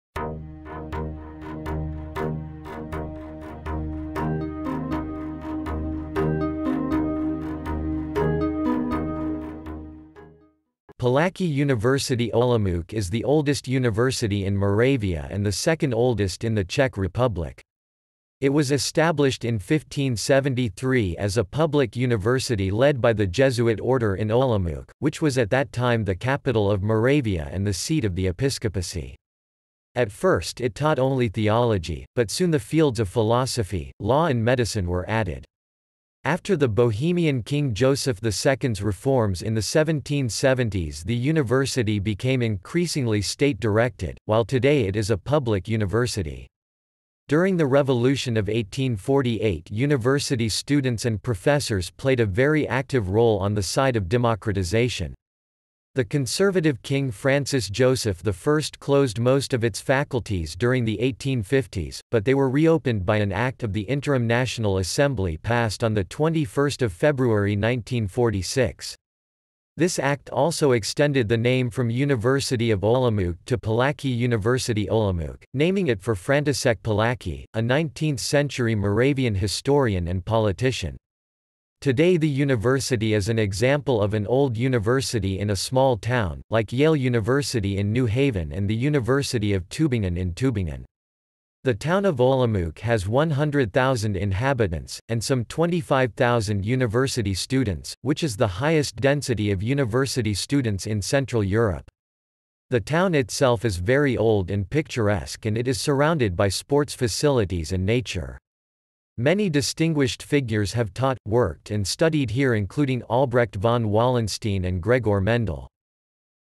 What is Palacký University?, Explain Palacký University, Define Palacký University
~~~ Palacký University ~~~

Title: What is Palacký University?, Explain Palacký University, Define Palacký University

Description: Palacký University Olomouc is the oldest university in Moravia and the second-oldest in the Czech Republic. It was established in 1573 as a public university led by the Jesuit order in Olomouc, which was at that time the capital of Moravia and the seat of the episcopacy. At first it taught only theology, but soon the fields of philosophy, law and medicine were added. After the Bohemian King Joseph II's reforms in the 1770s the university became increasingly state-directed, while today it is a public university. During the Revolution of 1848 university students and professors played a very active role on the side of democratisation. The conservative king Francis Joseph I closed most of its faculties during the 1850s, but they were reopened by an act of the Interim National Assembly passed on 21 February 1946. This act also extended the name from University of Olomouc to Palacký University Olomouc, naming it for František Palacký, a 19th-century Moravian historian and politician. Today the university is an example of an old university in a small town, like Yale University in New Haven and the University of Tübingen in Tübingen. The town of Olomouc has 100,000 inhabitants , and some 25,000 university students , which is the highest density of university students in Central Europe. The town itself is very old and picturesque and it is surrounded by sports facilities and nature. Many distinguished figures have taught, worked and studied here including Albrecht von Wallenstein and Gregor Mendel.